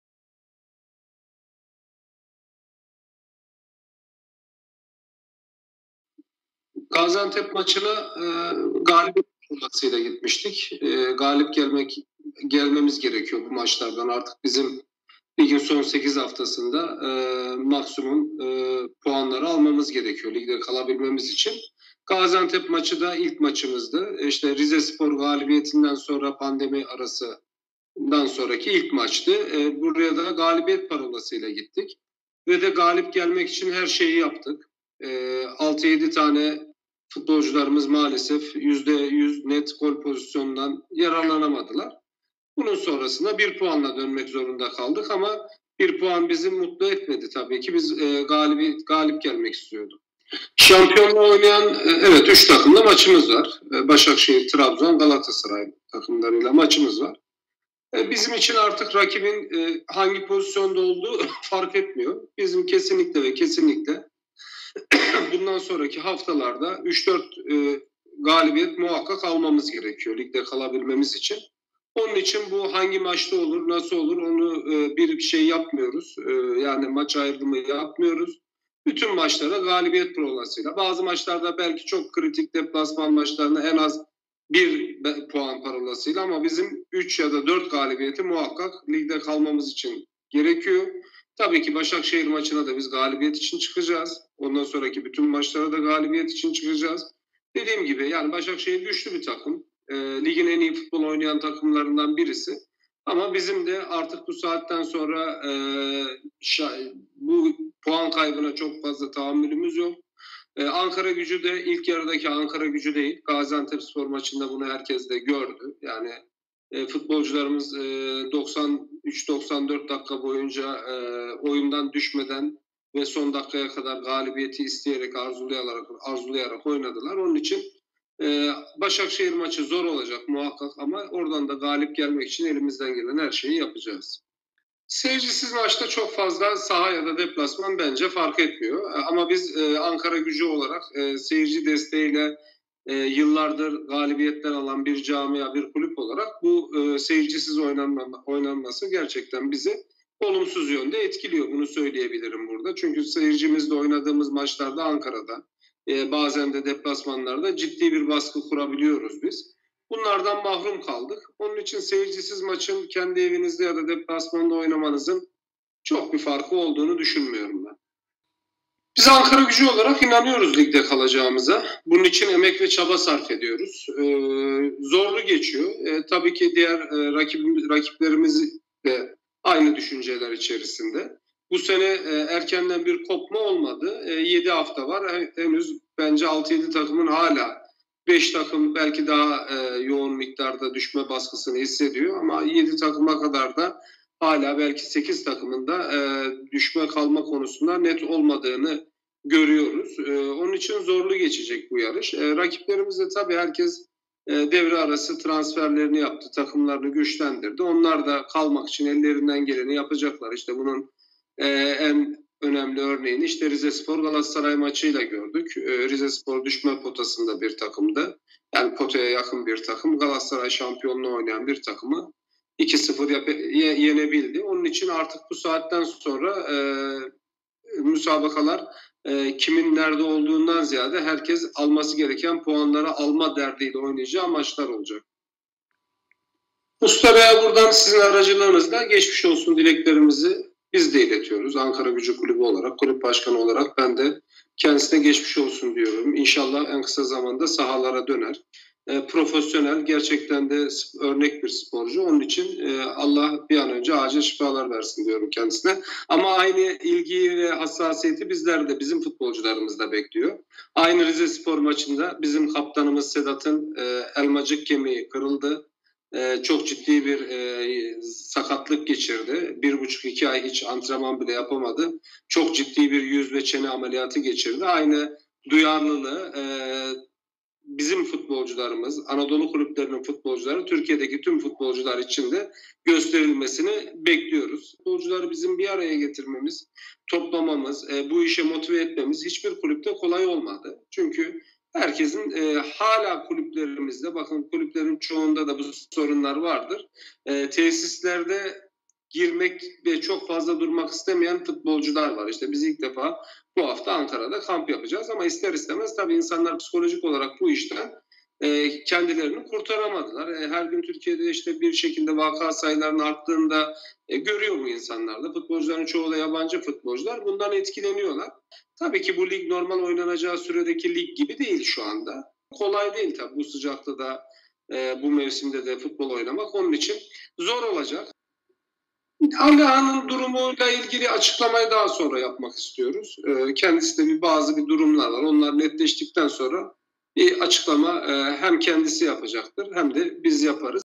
Başkan Fatih Mert: "Bütün maçlara galibiyet için çıkacağız" | MKE Ankaragücü
MKE Ankaragücü Kulübü Başkanı Fatih Mert, Gaziantep FK karşılaşmasında aldıkları 1 puandan mutlu olmadıklarını, kalan bütün maçlarında galibiyet hedefiyle sahaya çıkacaklarını söyledi.

Başkan Fatih Mert, sarı-lacivertli takımın ligde tutunma mücadelesi ve gündeme ilişkin AA muhabirine açıklamalarda bulundu.

Süper Lig'in 27. haftasındaki Gaziantep FK maçına galibiyet parolasıyla gittiklerini belirten Mert, "Bu maçta galip gelmemiz gerekiyordu. Artık ligde kalabilmemiz için maksimum puanları almalıyız. Gaziantep FK karşılaşması, yeni tip koronavirüs (Kovid-19) salgını nedeniyle verilen aradan sonraki ilk maçımızdı. Galibiyet parolasıyla gittik ve galip gelmek için her şeyi yaptık. Futbolcularımız maalesef 6-7 net gol pozisyonundan yararlanamadı. Bir puan bizi mutlu etmedi. Tabii ki galip gelmek istiyorduk." diye konuştu.

Ligin 28. haftasında 19 Haziran Cuma günü Medipol Başakşehir, 29. haftasında ise Trabzonspor'la karşılaşacaklarının hatırlatılması üzerine Başkan Fatih Mert, sözlerine şöyle devam etti:

"Önümüzde şampiyonluğa oynayan 3 takımla, Medipol Başakşehir, Trabzonspor ve Galatasaray'la maçımız var. Bizim için artık rakibin hangi pozisyonda olduğu fark etmiyor. Ligde kalabilmemiz için, bundan sonraki haftalarda 3-4 galibiyet alabilmemiz gerekiyor. Bütün maçlara galibiyet için çıkıyoruz. Tabii ki Medipol Başakşehir maçına da galibiyet için çıkacağız. Başakşehir güçlü, ligin en iyi futbol oynayan takımlarından birisi. Ancak bizim de bu saatten sonra puan kaybına tahammülümüz yok."

- "MKE Ankaragücü, ilk yarıdaki takım değil"

Başkan Mert, kadrolarına güvendiklerini dile getirerek, "MKE Ankaragücü ilk yarıdaki takım değil. Gaziantep FK maçında bunu herkes gördü. Futbolcularımız 93-94 dakika boyunca oyundan düşmeden, son dakikaya kadar galibiyeti isteyerek, arzulayarak oynadılar. Onun için Medipol Başakşehir maçı zor olacak ama galip gelmek için elimizden gelen her şeyi yapacağız." ifadelerini kullandı.

Seyircisiz maçlarda deplasman ya da iç saha farkı oluşmadığını anlatan Fatih Mert, "MKE Ankaragücü, seyirci desteğiyle galibiyetler alan bir camia. Maçların seyircisiz oynanması bizi gerçekten olumsuz yönde etkiliyor. Seyircili maçlarımızda ciddi baskı kurabiliyoruz. Bundan mahrum kaldık." şeklinde konuştu.

Süper Lig'de 24 puanla 17. sırada ve küme düşme hattında yer aldıklarını vurgulayan Mert, şu ifadeleri kullandı:

"MKE Ankaragücü olarak ligde kalacağımıza inanıyor, bunun için emek ve çaba sarf ediyoruz. Zorlu geçiyor. Tabii ki rakiplerimiz de aynı düşünce içerisinde. Bu sene erken bir kopma olmadı. Önümüzde 7 maç daha var. Belki 5 takım daha yoğun hissediyor ama 6-7 takımın hala kümede kalma konusunda net olmadığını görüyoruz. Onun için bu yarış zorlu geçecek. Tabii herkes devre arası transferlerini yaptı, takımlarını güçlendirdi. Telikeli bölgede bulunan tüm takımlar ligde kalmak için ellerinden gelen çabayı gösterecek. Bunun önemli örneğini Çaykur Rizespor-Galatasaray maçında gördük. Çaykur Rizespor düşme potasına yakın bir takımdı ve şampiyonluğa oynayan Galatasaray'ı yenebildi. Onun için artık bu saatten sonra herkes ihtiyacı olan puanları alma derdiyle oynayacak."

Koronavirüs nedeniyle verilen arada futbolcuların yeterli şekilde hazırlanıp hazırlanamadığına yönelik Fatih Mert, "Koronavirüs sürecinde futbolcuları toplamamız, antrenmanlara başlamamız kolay olmadı. Hala çok fazla tesislere girmek ve orada durmak istemeyen futbolcular var. Biz ilk defa bu hafta Ankara'da kamp yapacağız ama ister istemez insanlar psikolojik olarak bu işten kendilerini kurtaramadılar. Türkiye'de vaka sayılarında artış olduğunu görüyorlar ve bundan etkileniyorlar. Şu anda lig normal şekilde oynanacak gibi değil. Bu mevsimde, bu şartlarda futbol oynamak kolay değil." açıklamasında bulundu.

Savunma oyuncusu Alihan Kubalas'la yolları ayırdıklarına ilişkin haberler için Fatih Mert, "Alihan'ın durumuyla ilgili açıklamayı daha sonra yapacağız. Kendisinin bazı durumları var. Onlar netleştikten sonra hem kendisi açıklama yapacaktır hem de biz yaparız." diye konuştu.

- "Aynı hassasiyeti bizim futbolcularımız da bekliyor"

Başkan Fatih Mert, Galatasaray file bekçisi Fernando Muslera'nın Çaykur Rizespor deplasmanında ayağının kırılmasıyla ilgili geçmiş olsun dileklerini aktarırken, sözlerini şöyle tamamladı:

"MKE Ankaragücü Kulübü olarak bir kez daha Fernando Muslera'ya geçmiş olsun dileklerimizi iletiyoruz. İnşallah en kısa zamanda sahalara döner. Gerçekten de örnek bir sporcu. Aynı ilgiyi, hassasiyeti